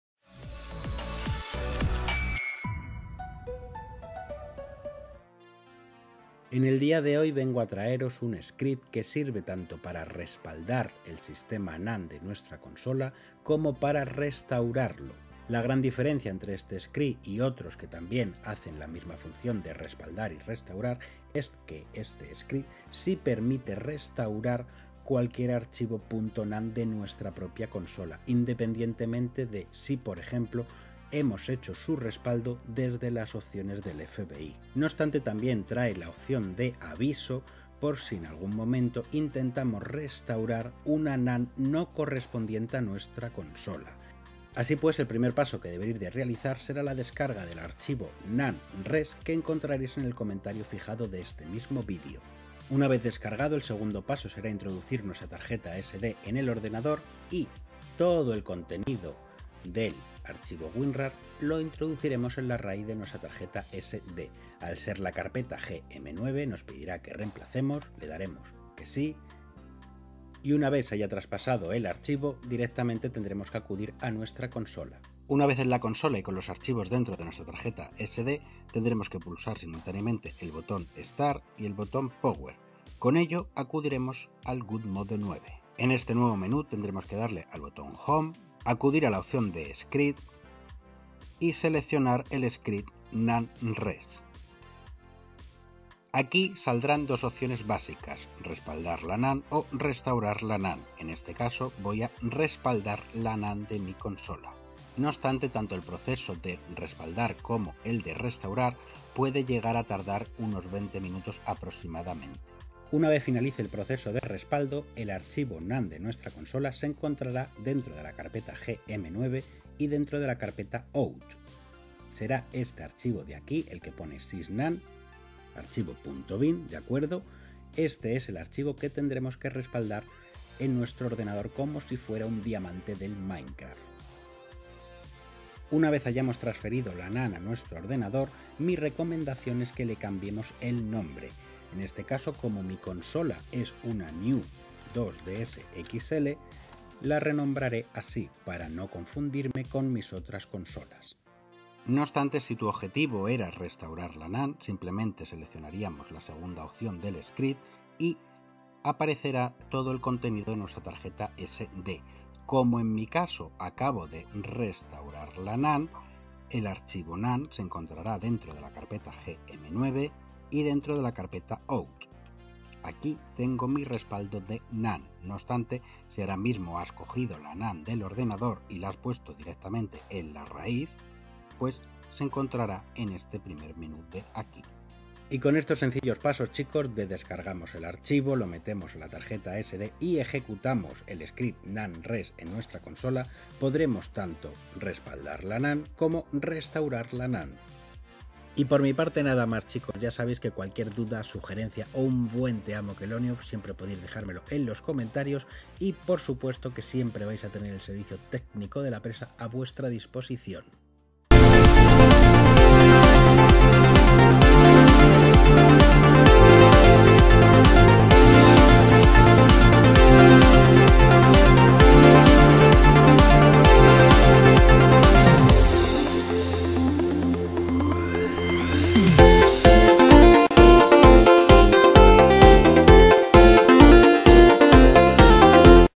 ¡Respaldar NAND en 3DS y 2DS en 2026 SIN RIESGO de BRICK! 🎮 Tutorial FÁCIL con Nand-rex
*Vídeo creado por* @Kelonio3DS

*PASOS A SEGUIR +  RESUMEN DEL PROCESO*
00:00 Introducción
00:44 Descargar Archivos NAND REX
01:14 Ejecutamos el Script.gm9
01:35 Respaldar NAND
01:52 Archivo NAND en el PC
02:31 Restaurar NAND
03:04 Despedida
03:32 Mención Suscriptores
*El vídeo enseña cómo respaldar y restaurar la NAND de una 3DS* con el script nandrex.gm9, compatible con cualquier NAND de la consola (00:00:06-00:00:46).

*Pasos:*
*1.* Descarga y preparación (00:00:46-00:01:16): Descarga "nandrex.gm9" del comentario fijado, extrae el contenido del WinRAR a la raíz de la SD (reemplaza "gm9").

*2.* Ejecución (00:01:16-00:03:07): Enciende con Start+Power, en GodMode9 (Home - Scripts - "Nand-rex"), elige "Respaldar" o "Restaurar" (20 min aprox.). Respaldo va a "gm9/out/sysnand.bin"; para restaurar, selecciona el archivo NAND en SD.

*3.* Post-proceso (00:03:07-00:03:30): Guarda "sysnand.bin" en el PC (renómbralo para identificar tu consola, ej. "New"). Para restaurar, usa un NAND previo desde "gm9/out" o raíz.

*Nota* (00:03:30-00:03:30): Advierte si el NAND no coincide. Soporte en comentarios o "La Presa".

*RECUERDA QUE PARA UNA MAYOR COMPRENSIÓN PASO A PASO PUEDES GUIARTE CON LOS CAPÍTULOS Y CON LA TRANSCRIPCIÓN*